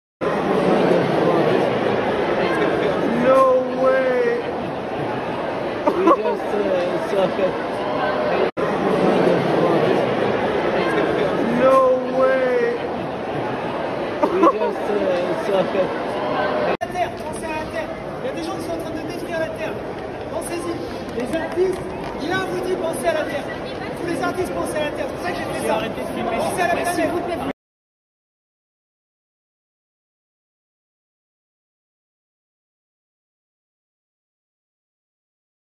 Mona Lisa smeared with cake by man disguised as elderly woman
ART lovers have been left stunned as a man disguised as an old woman leapt from a wheelchair and threw a cake at the Mona Lisa.

Footage of the incident at the Louvre shows the world-famous painting smeared with cake by an eco-vandal before he is dragged away by security guards, as stunned onlookers watch on.

Luckily, no damage was done to the masterpiece as it is protected by a bulletproof screen.

The eco-hooligan, who was in a wheelchair and dressed in a wig and lipstick, attacked the painting in a climate protest - although it is unclear why he chose the Mona Lisa as his target.

Following the cake-attack at the Louvre, in Paris, France, the perpetrator yelled: "Think about the Earth. People are in the process of destroying the Earth!

"Artists think about the Earth, that's why I did this. Think of the planet!"

Witnesses said that the man rolled past the painting in the wheelchair before jumping to his feet and launching a cake at the portrait.

The man then allegedly threw a bouquet of roses into the air before being dragged away by security guards.

Baffled onlookers took to social media to share their shock, with one tweeting: "Maybe this is just nuts to me but an man dressed as an old lady jumps out of a wheel chair and attempted to smash the bullet proof glass of the Mona Lisa.

Continue reading
Mona Lisa protest – Shock moment eco-warrior disguised as wheelchair-bound old woman smears CAKE on masterpiece